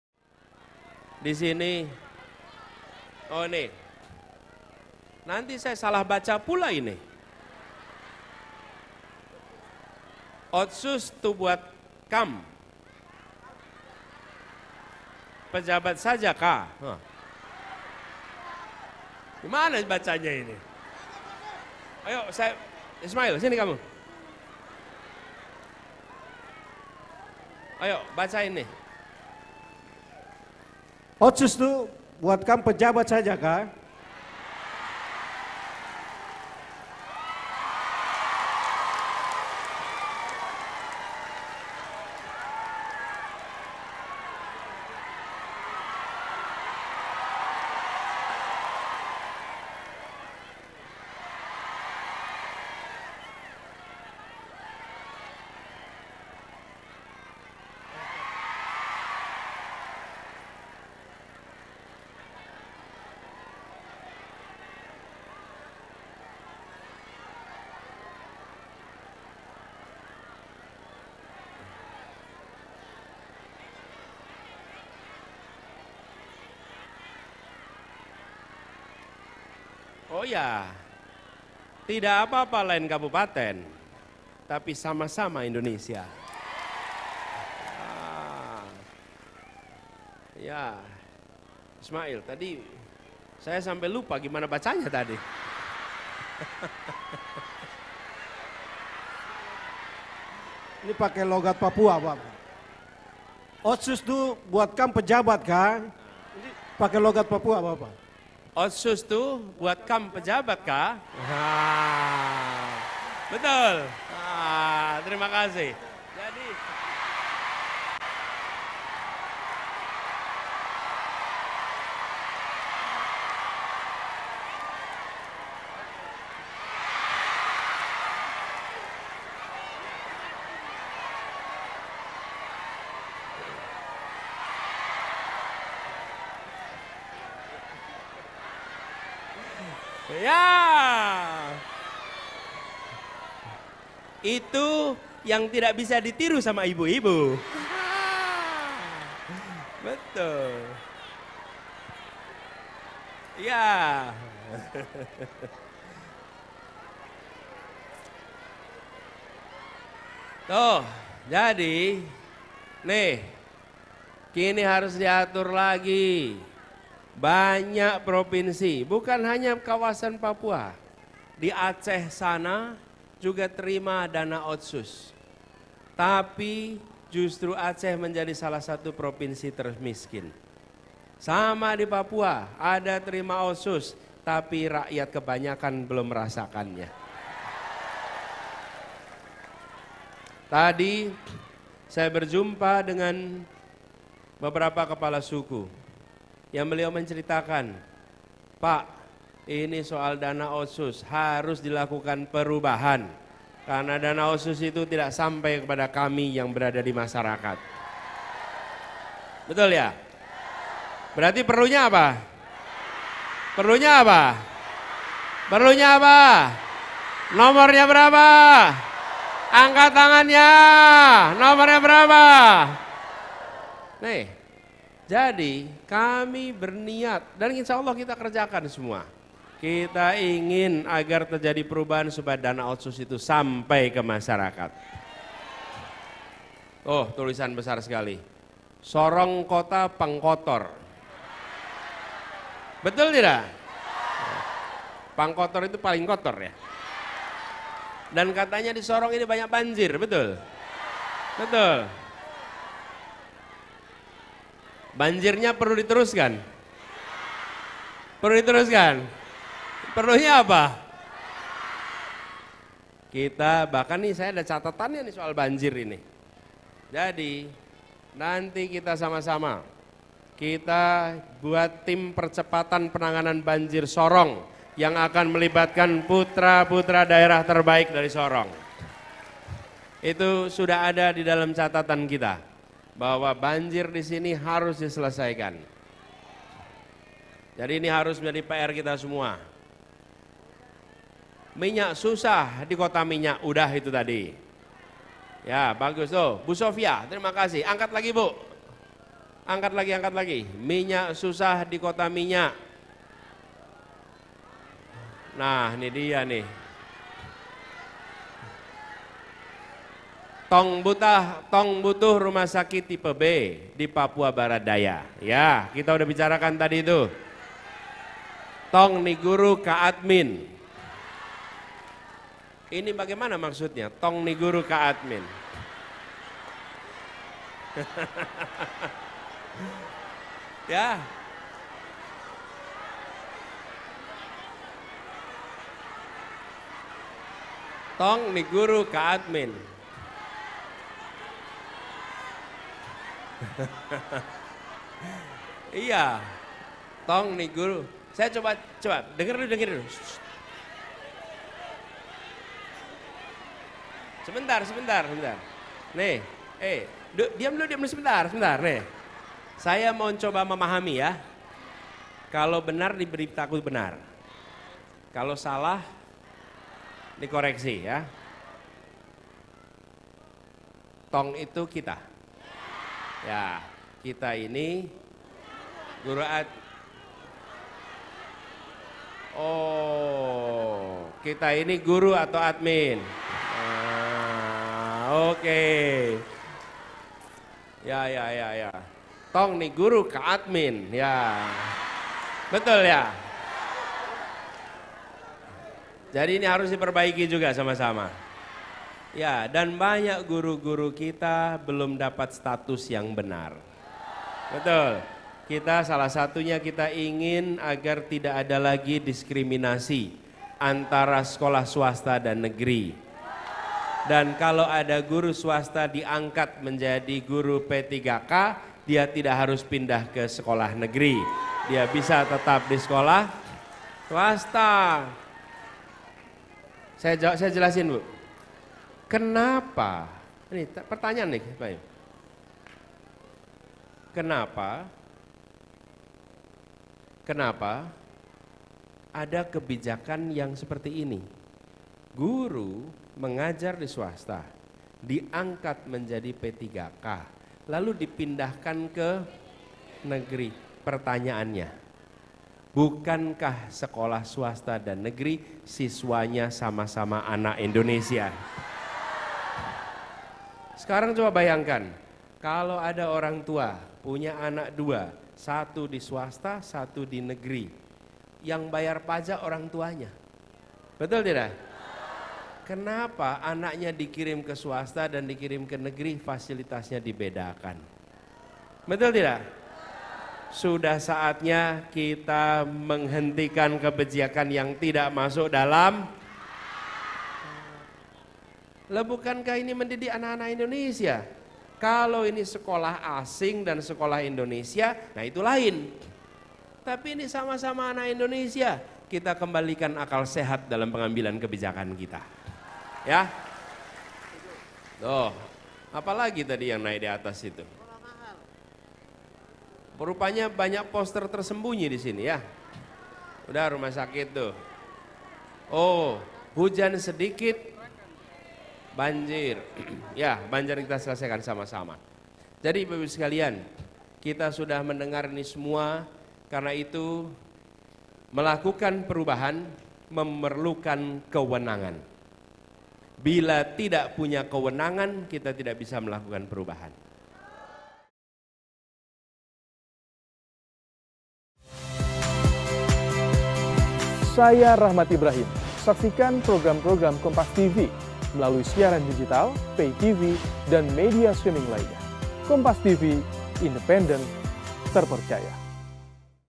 Anies Soroti Ketimpangan di Papua, Singgung Dana Otonomi Khusus Harus Bisa Dirasakan Manfaatnya
KOMPAS.TV - Capres Nomor Urut 1, Anies Baswedan menghadiri acara simpatisan Anies-Muhaimin (AMIN) yang digelar di Sorong, Papua Barat pada Selasa, (16/1/2024).

Dalam acara tersebut, Anies sempat menyoroti kebijakan dana Otonomi Khusus (Otsus) di Papua.

Terdapat momen yang menarik dalam acara tersebut, salah satu simpatisan yang hadir memeluk dirinya

Momen itu terjadi saat Anies meminta membacakan aspirasi dengan menggunakan bahasa Papua soal dana Otsus kepada salah satu simpatisan.